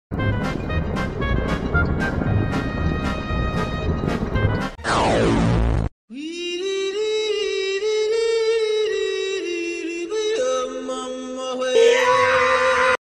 SCP-173 is a Peanut (SCP Animation)
He has evolved into his true form... the peanut.

Programs used: Animate, Paint tool sai